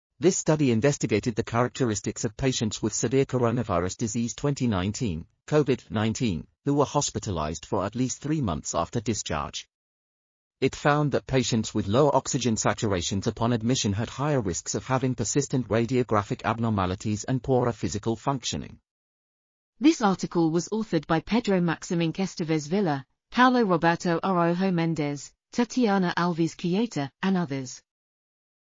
Oximetry at admission as a predictor of tomographic and functional impairment after 3... | RTCL.TV
### Keywords ###

### Article Attribution ###
Title: Oximetry at admission as a predictor of tomographic and functional impairment after 3–6 months in hospitalized patients with COVID-19
Authors: Pedro Maximink Esteves Villar, Paulo Roberto Araújo Mendes, Tatiana Alves Kiyota, Henrique Alcântara Engleitner, Carolina Salem Tamesawa, Mayara Magalhães Morello, Nayara Nobre Basso, Lucas Fileti Arruda, Bruna Scharlack Vian, Lígia dos Santos Roceto Ratti, Laís Bacchin de Oliveira, Ana Lucia Cavallaro Baraúna Lima, Hugo Dugolin Ceccato, Julian Furtado Silva, Wander de Oliveira Villalba, Lucieni de Oliveira Conterno, Mariângela Ribeiro Resende ,and Mônica Corso Pereira
Publisher: SAGE Publishing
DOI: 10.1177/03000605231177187

### Video Timestamps ###
0:00:00 - Summary
0:00:19 - Title
0:00:28 - End